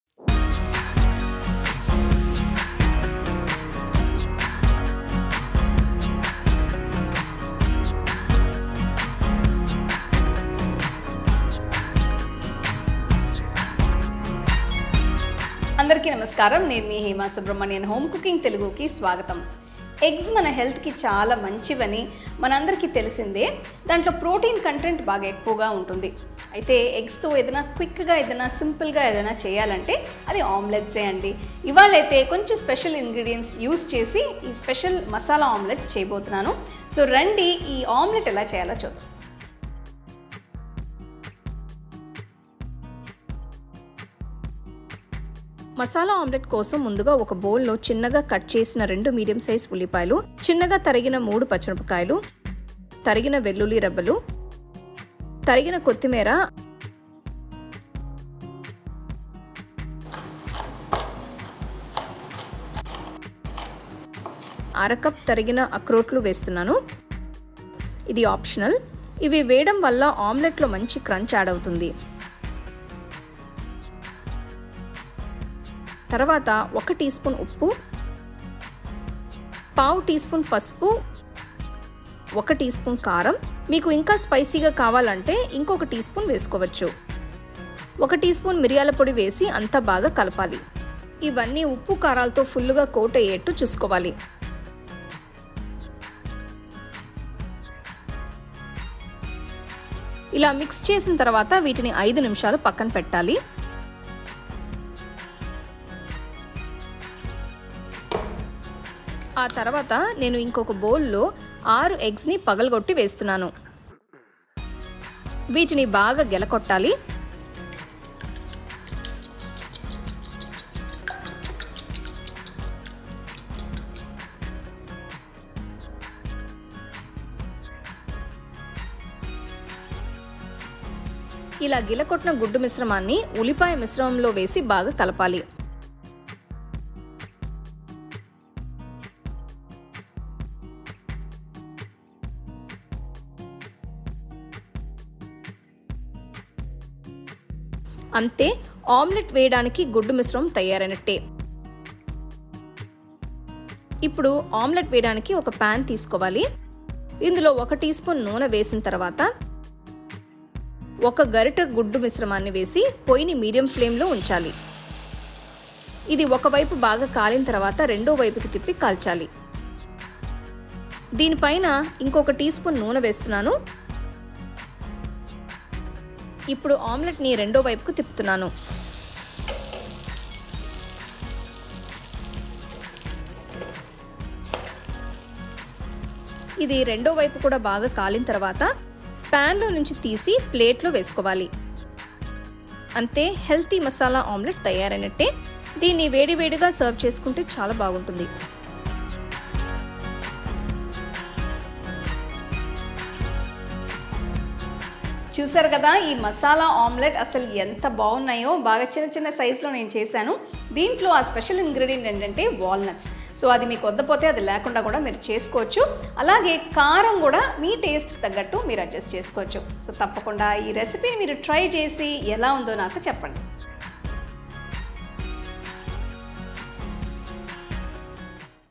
మసాలా ఆమ్లెట్ | Masala Omelette | Breakfast Recipes | Egg Recipes | Healthy Breakfast Ideas
మసాలా ఆమ్లెట్ | Masala Walnut Omelette | Breakfast Recipes | Egg Recipes | Healthy Breakfast Ideas | Walnut Recipe

మసాలా ఆమ్లెట్ చాలా తేలికైన బ్రేక్ఫాస్ట్, ఇంకా చాలా హెల్తీ కూడా. దీనిలో అక్రోట్లు వేయడం వలన ఈ ఆమ్లెట్లో మంచి రుచి, క్రంచ్ కూడా జోడవుతాయి. తప్పకుండా ఈ మసాలా ఆమ్లెట్ను ట్రై చేసి ఎంజాయ్ చేయండి.

తయారుచేయడానికి: 20 నిమిషాలు
వండటానికి: 25 నిమిషాలు
సెర్వింగులు: 6

కావలసిన పదార్థాలు:

ఉల్లిపాయలు - 2 (చిన్నగా తరిగినవి)
పచ్చిమిరపకాయలు - 3 (చిన్నగా తరిగినవి)
తరిగిన వెల్లుల్లి రెబ్బలు
తరిగిన కొత్తిమీర
అక్రోట్లు - 1 / 2 కప్పు (తరిగినవి)
ఉప్పు - 1 టీస్పూన్
పసుపు - 1 / 4 టీస్పూన్
కారం - 1 టీస్పూన్
మిరియాల పొడి - 1 టీస్పూన్
గుడ్లు - 6
నూనె - 1 టీస్పూన్

తయారుచేసే విధానం:

ముందుగా ఒక పెద్ద బౌల్లో తరిగిన ఉల్లిపాయలు, తరిగిన పచ్చిమిరపకాయలు, తరిగిన వెల్లుల్లి తరిగిన కొత్తిమీర, తరిగిన ఆక్రోట్లు, ఉప్పు, కారం, పసుపు, మిరియాల పొడి వేసి బాగా కలిపి కనీసం ఐదు నిమిషాల పాటు పక్కన పెట్టాలి

ఇప్పుడు ఇంకొక బౌల్లో గుడ్లను పగలకొట్టి వేసి, బాగా గిలక్కొట్టాలి

ఈ గిలకొట్టిన గుడ్లను ఉల్లిపాయ మిశ్రమంలో వేసి కలపాలి

ఇప్పుడు ఒక ప్యాన్లో నూనె వేసి, అందులో గరిటెతో గుడ్డు మిశ్రమాన్ని ఆమ్లెట్లా వేయాలి

ఆమ్లెట్ ఒకవైపు కాలిన తరువాత, పైన కొద్దిగా నూనె వేసి, ఇంకొక వైపుకి తిప్పి కాల్చాలి

అంతే, మసాలా ఆమ్లెట్ తయారైనట్టే, వీటిని వెంటనే వేడిగా సర్వ్ చేసుకుంటే చాలా బాగుంటాయి